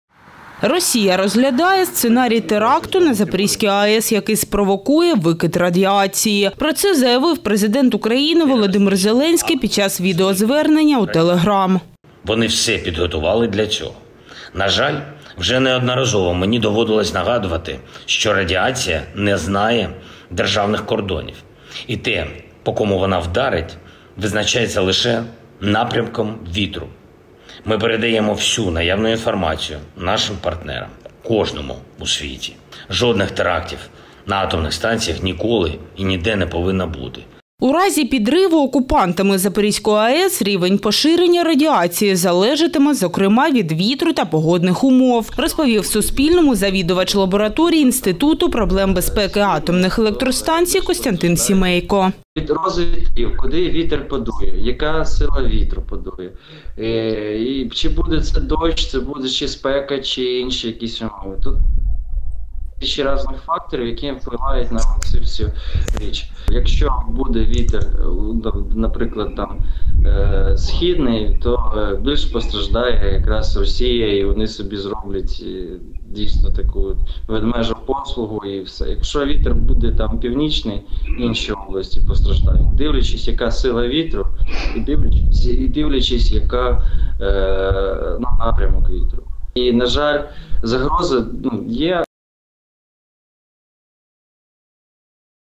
Росія розглядає сценарій теракту з викидом радіації на ЗАЕС — Зеленський | Новини
Росія розглядає сценарій теракту на Запорізькій АЕС, який спровокує викид радіації –  про це заявив Президент України Володимир Зеленський під час відеозвернення у «Телеграм».